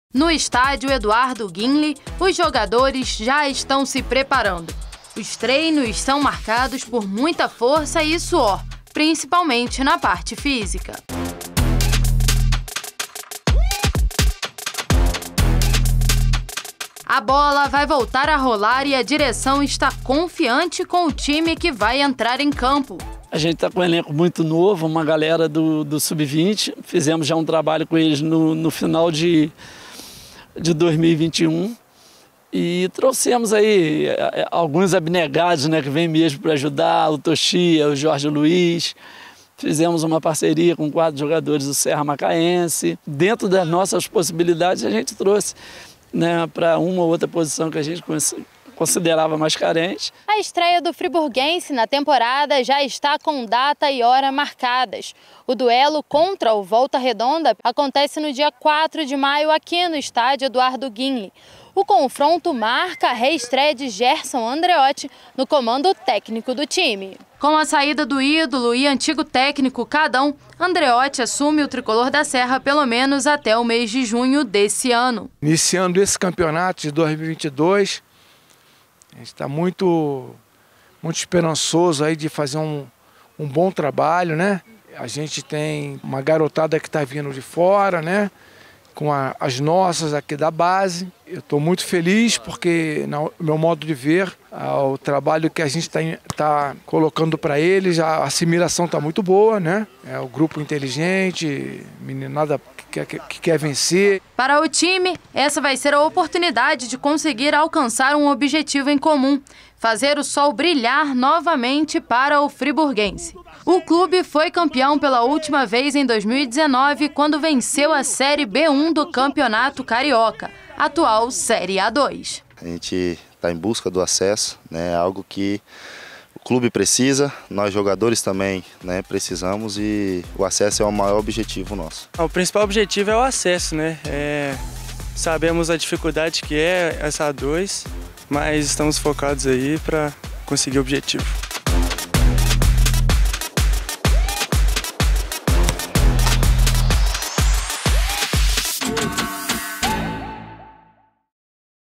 Frizão se prepara para encarar o Volta Redonda pela Taça Santos Dumont|| portalmultiplix.com
O Friburguense, time de futebol de Nova Friburgo, na Região Serrana do Rio, encara o Volta Redonda, no próximo dia 4, pela Taça Santos Dumont. O tricolor da Serra aposta em treinos diários e a experiência do técnico Gerson Andreotti para buscar o acesso à Série A1 do Campeonato Carioca.  O clube foi campeão em 2019, quando venceu a série B1 da competição, atual série A2. Confira na reportagem acima!